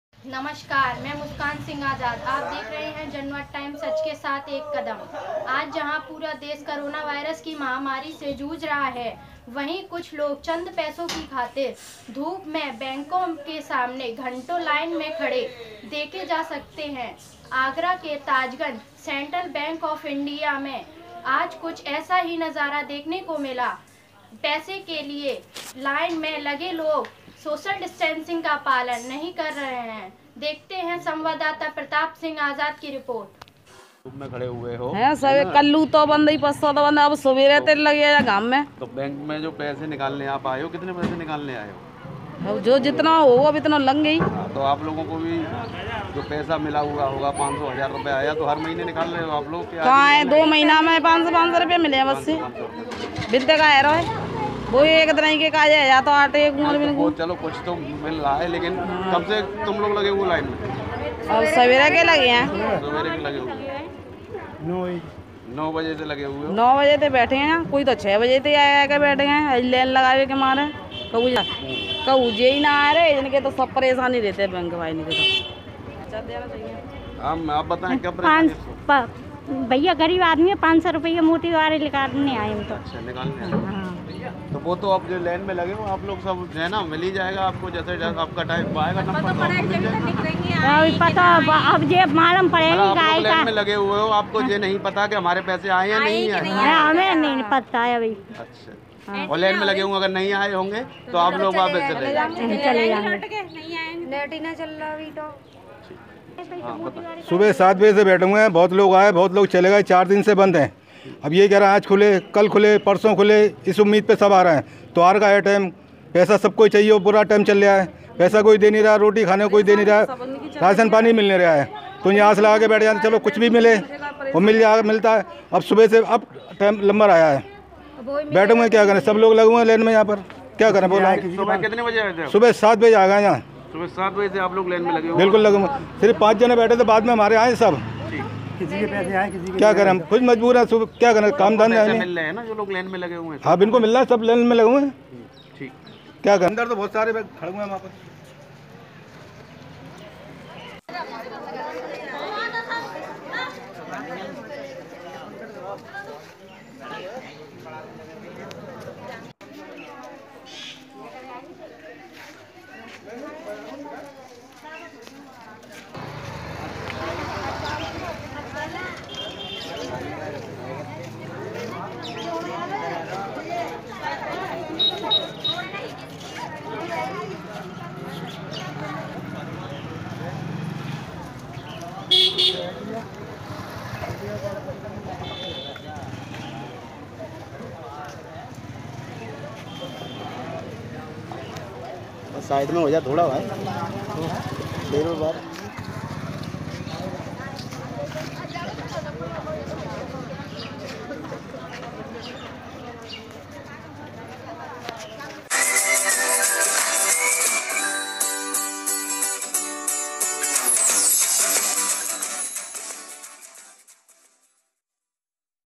सेंट्रल बैंक ऑफ इंडिया ताजगंज में पैसे के लिए धूप में खड़े लोगों ने उड़ाई सोशल डिस्टेंस की धज्जियां
Janvad Times : Agra UP : सेंट्रल बैंक ऑफ इंडिया ताजगंज में पैसे के लिए धूप में खड़े लोगों ने उड़ाई सोशल डिस्टेंस की धज्जियां